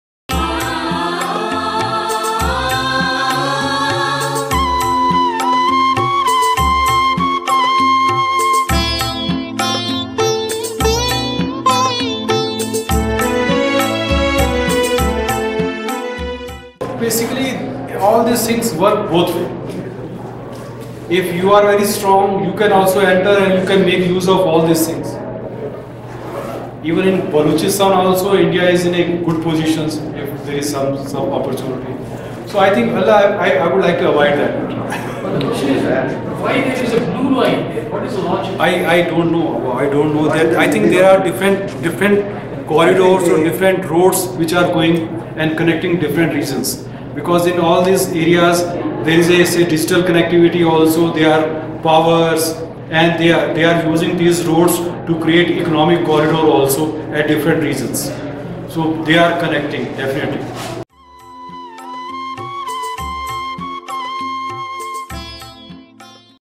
Why is India opposing China Belt Road Initiative while most of its Strategy is useful to all?
Bhartiya Vichar Manch. Los Angeles. Monthly Lecture Series.

Topic: China's Belt Road Initiative (OBOR): It's Strategic, Economic and
Security Implications for India

Speaker:  Prof. Vijay Kumar Kaul
Professor Vijay Kumar Kaul Ph.D.
 Professor of Business Economics
 Head and Dean, Faculty of Social Sciences and Humanities, University of Delhi

Talk Abstract:
 China's $1 trillion Belt and Road Initiative (BRI), earlier known as OBOR (one Belt one Road), aims to improve its connectivity with as many as 65 countries, including Europe. The initiative has been equated with Marshal Plan or 21st Century Silk Road etc. However, concerns have been raised in India and globally whether BRI is a form of economic imperialism that gives China too much leverage over other countries.

About Speaker:
Prof. Vijay Kumar Kaul is the head of Department of Business Economics and Dean of Faculty of Applied Social Sciences and Humanities at the prestigious University of Delhi, India. He is a well-known expert in Geo-economics and Geopolitics, Economic Transition and Performance, Knowledge Economy in Global Context, Public Policy and Business, India’s Diversity and Globalization.